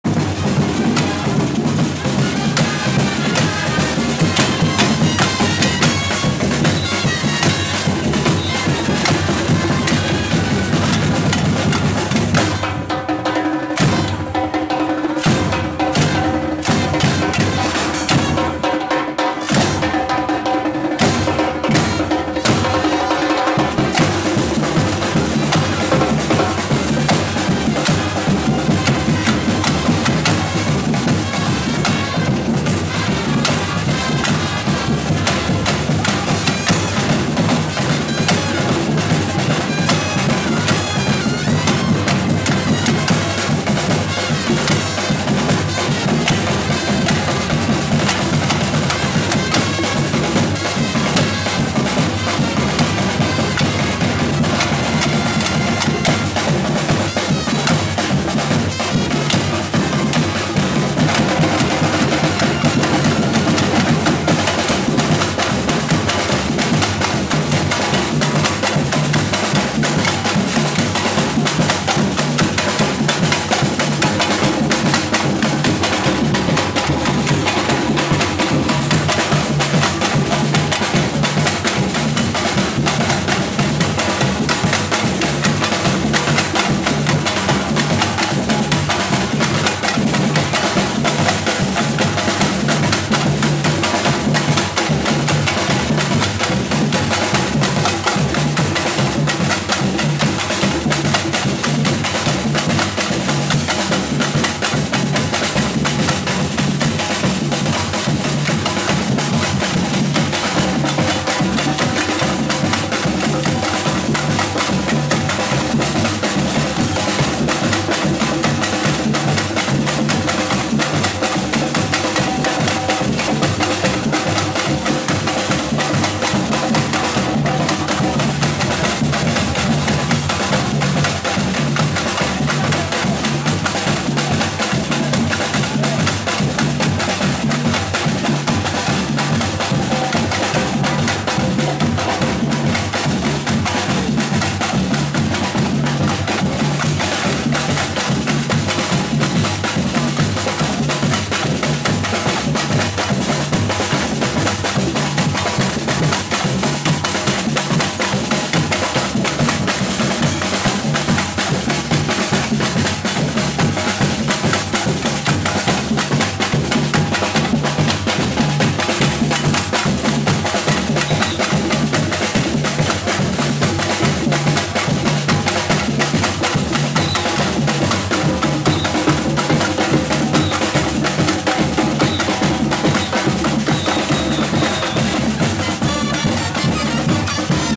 CARNAVALADA BARCELONETA 2020 - BARNE BATUQUE
NOMBRE DEL GRUPO:BARNE BATUQUE
NOMBRE DEL EVENTO:CARNAVALADA BARCELONETA
FECHA DEL EVENTO:01/03/2020
LUGAR DEL EVENTO: BARCELONETA
FILMADO POR: BATUCHARLY
PARTNER: MACHINIMA